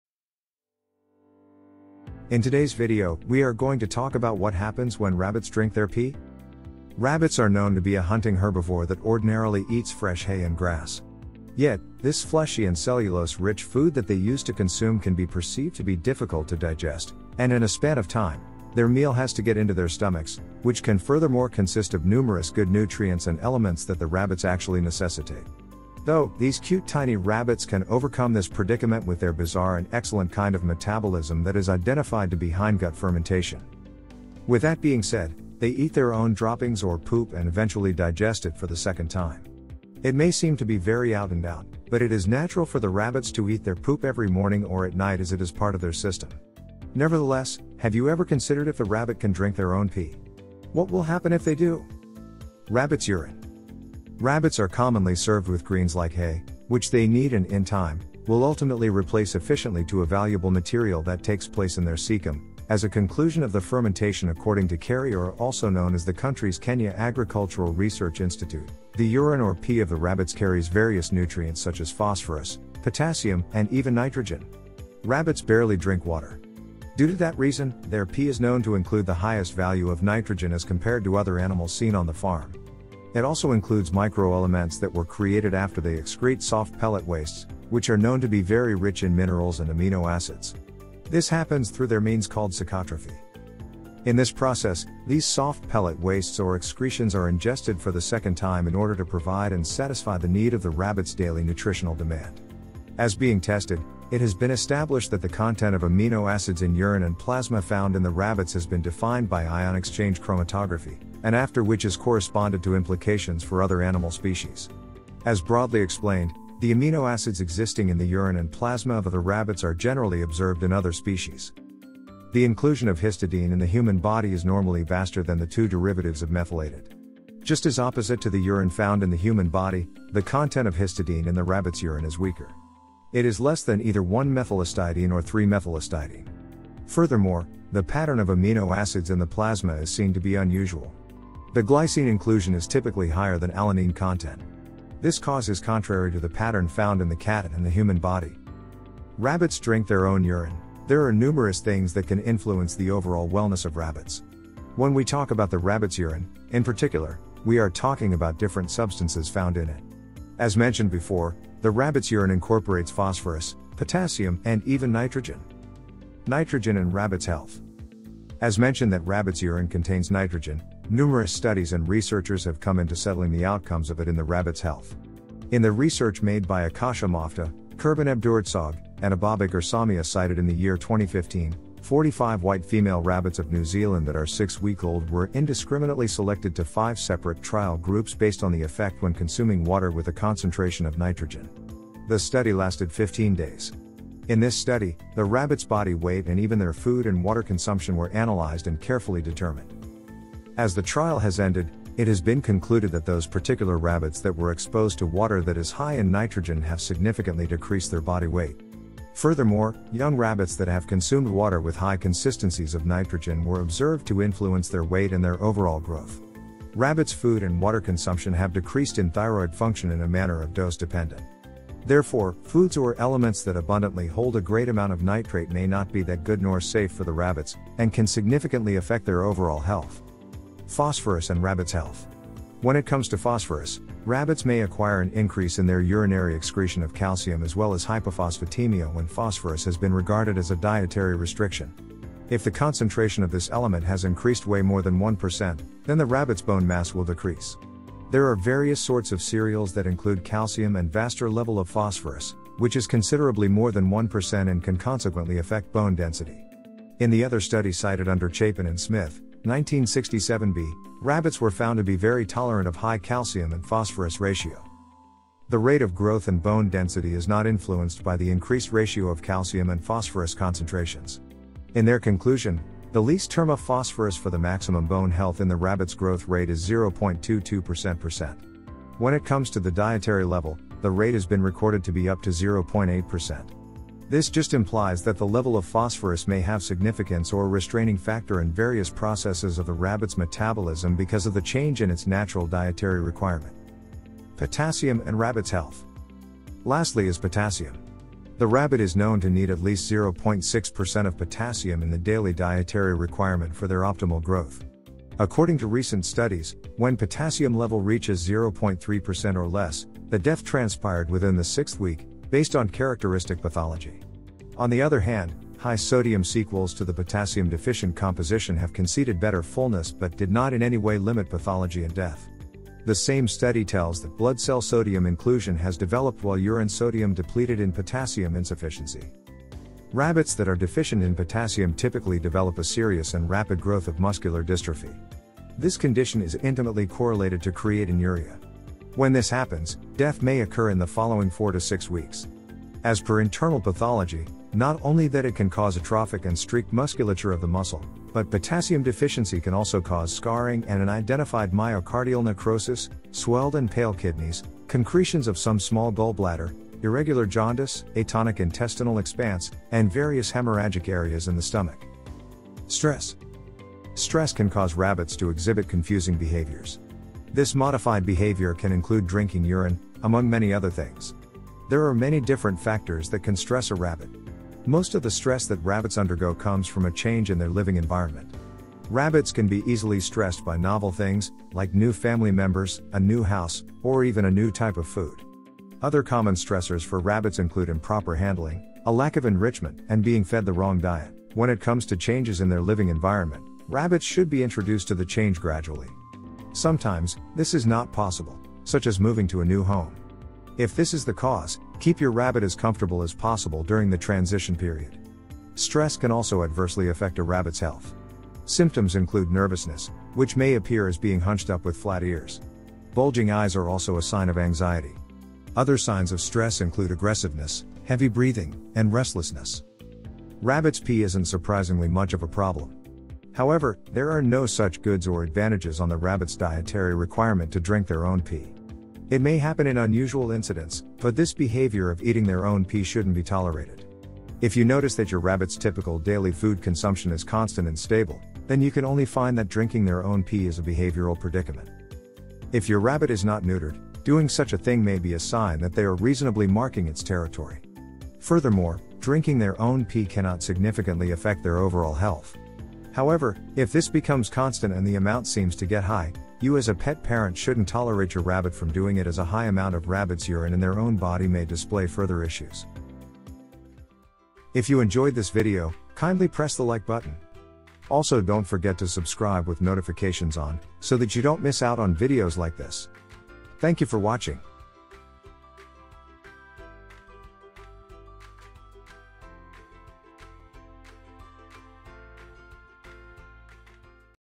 Rabbit 101: Rabbits Drink Their Pee
In today's video, we are going to talk about What Happens When Rabbits Drink Their Pee?

Rabbits are known to be a hunting herbivore that ordinarily eats fresh hay and grass. Yet, this fleshy and cellulose-rich food that they use to consume can be perceived to be difficult to digest, and in a span of time, their meal has to get into their stomachs, which can furthermore consist of numerous good nutrients and elements that the rabbits actually necessitate. Though, these cute tiny rabbits can overcome this predicament with their bizarre and excellent kind of metabolism that is identified to be hindgut fermentation.

With that being said, they eat their own droppings or poop and eventually digest it for the second time. It may seem to be very out-and-out, but it is natural for the rabbits to eat their poop every morning or at night as it is part of their system. Nevertheless, have you ever considered if the rabbit can drink their own pee? What will happen if they do?

Rabbit’s Urine.

Rabbits are commonly served with greens like hay; which they need and in time, will ultimately replace efficiently to a valuable material that takes place in their caecum, as a conclusion of the fermentation According to KARI or also known as the country’s Kenya Agricultural Research Institute, the urine or pee of the rabbits carries various nutrients such as phosphorus, potassium, and even nitrogen. Rabbits barely drink water. Due to that reason, their pee is known to include the highest value of nitrogen as compared to other animals seen on the farm. It also includes microelements that were created after they excrete soft pellet wastes, which are known to be very rich in minerals and amino acids. This happens through their means called caecotrophy.

In this process, these soft pellet wastes or excretions are ingested for the second time in order to provide and satisfy the need of the rabbit’s daily nutritional demand. As being tested, it has been established that the content of amino acids in urine and plasma found in the rabbits has been defined by ion-exchange chromatography, and after which is corresponded to implications for other animal species. As broadly explained, the amino acids existing in the urine and plasma of the rabbits are generally observed in other species.

The inclusion of histidine in the human body is normally vaster than the two derivatives of methylated. Just as opposite to the urine found in the human body, the content of histidine in the rabbit’s urine is weaker. It is less than either one methylhistidine or 3-methylhistidine. Furthermore, the pattern of amino acids in the plasma is seen to be unusual. The glycine inclusion is typically higher than alanine content. This cause is contrary to the pattern found in the cat and the human body.

Rabbits drink their own urine.

There are numerous things that can influence the overall wellness of rabbits. When we talk about the rabbit’s urine, in particular, we are talking about different substances found in it. As mentioned before, the rabbit’s urine incorporates phosphorus, potassium, and even nitrogen.

Nitrogen and Rabbit’s Health.

As mentioned that rabbit’s urine contains nitrogen, numerous studies and researchers have come into settling the outcomes of it in the rabbit’s health. In the research made by Akasha Moftah, Kerban Abdurzag, and Abobaker Somia cited in the year 2015, 45 white female rabbits of New Zealand that are six-week-old were indiscriminately selected to 5 separate trial groups based on the effect when consuming water with a concentration of nitrogen. The study lasted 15 days. In this study, the rabbit’s body weight and even their food and water consumption were analyzed and carefully determined.

As the trial has ended, it has been concluded that those particular rabbits that were exposed to water that is high in nitrogen have significantly decreased their body weight. Furthermore, young rabbits that have consumed water with high consistencies of nitrogen were observed to influence their weight and their overall growth. Rabbit’s food and water consumption have decreased in thyroid function in a manner of dose-dependent. Therefore, foods or elements that abundantly hold a great amount of nitrate may not be that good nor safe for the rabbits, and can significantly affect their overall health.